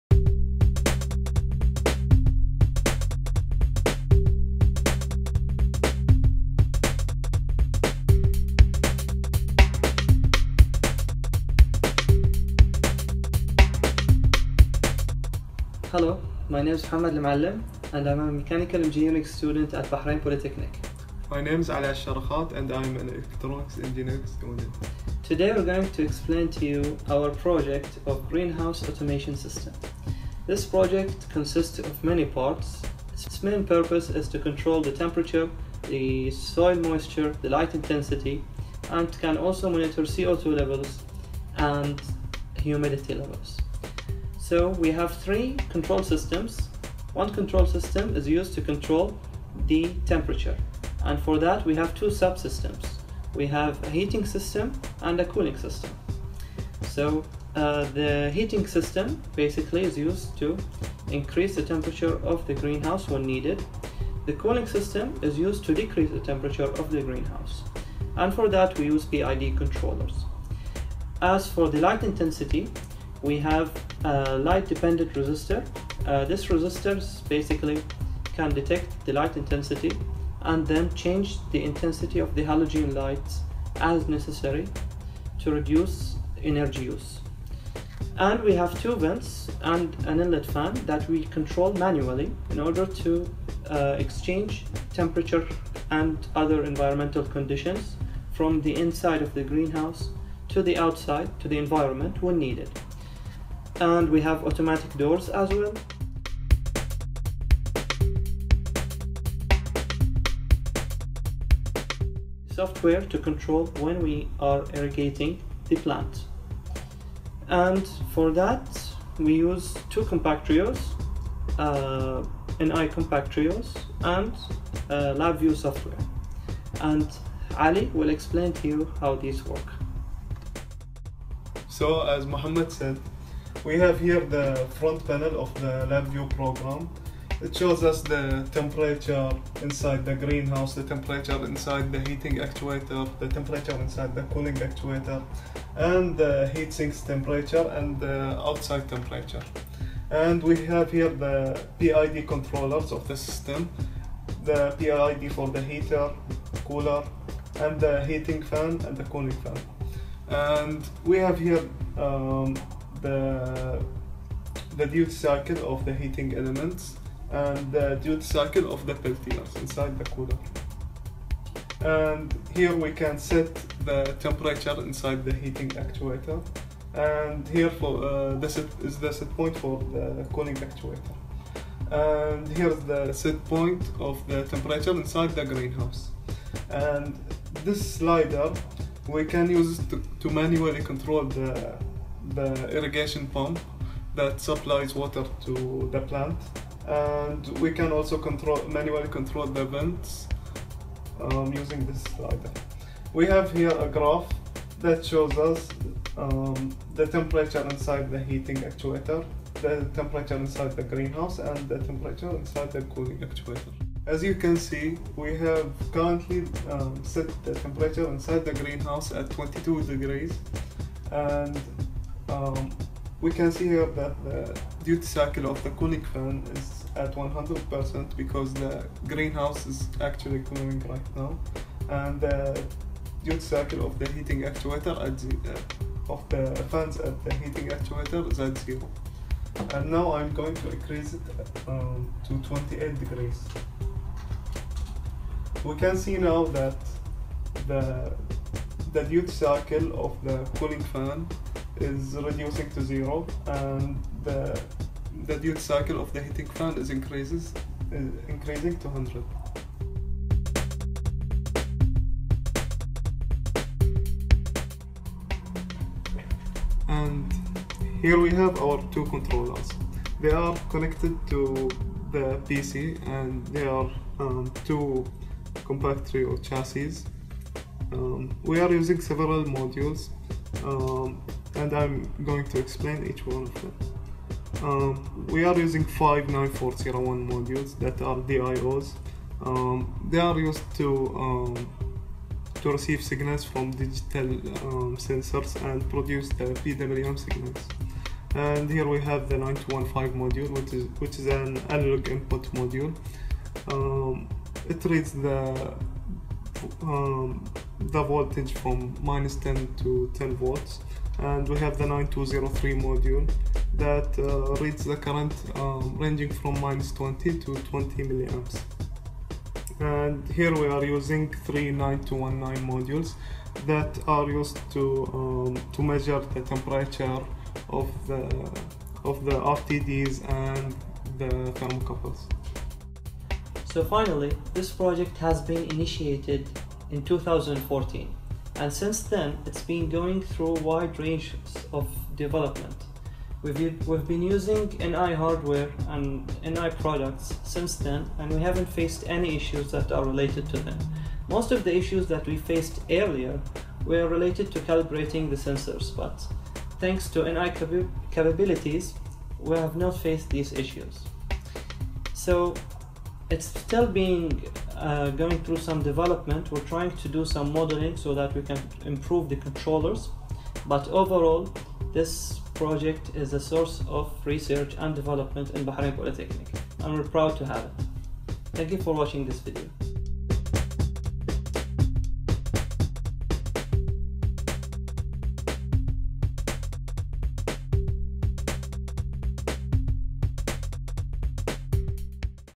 Environmental Control of a Greenhouse System using NI Embedded Systems Technology
The greenhouse prototype system was built using commercial embedded systems technology. It was developed and instrumented with appropriate sensors to measure various environmental variables like the temperature, the light intensity, the soil moisture, the air humidity and CO2 concentration. These measurements are provided to the control algorithm which is implemented on a commercial embedded system and manipulates various actuators like, a heating and cooling actuator, fans, lights, irrigation system, and louvers in order to achieve the desired set-points, as specified by the user through a Human-Machine Interface implemented in LabView software.

The purpose of this work is to enhance research related to the accurate environmental control of greenhouse systems in order to minimize energy and water consumption and to develop a robust educational platform for teaching control system design, analysis, instrumentation and embedded systems development at the Engineering School of Bahrain Polytechnic.